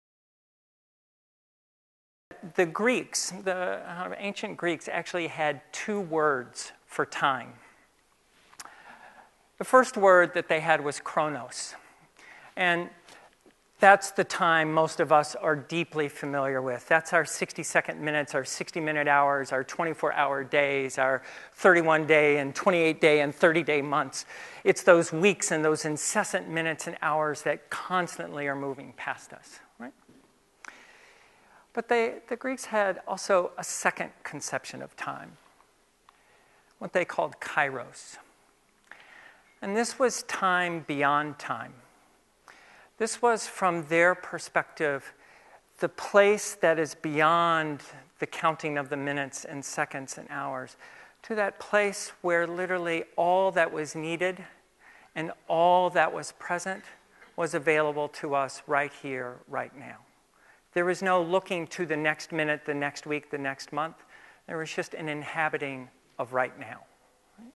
John Ott and time
HC Link gathering keynote speaker John Ott outlines the differences
between Kronos and Kairos time.